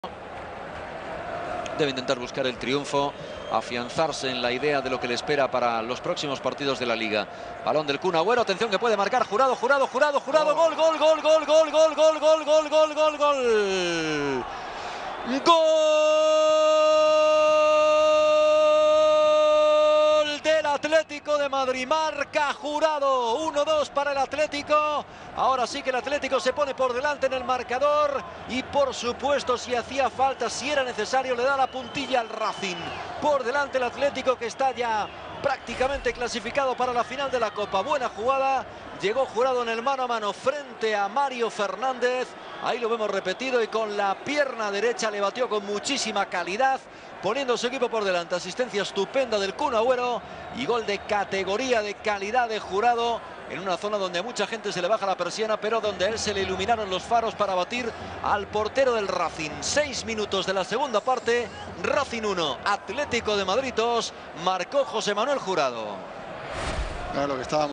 Gol Jurado Racing 1-2 Atletico, semifinales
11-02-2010. Semifinales Copa del Rey Racing - Atletico de Madrid. Pared entre Jurado y Agüero que finaliza con éxito el primero.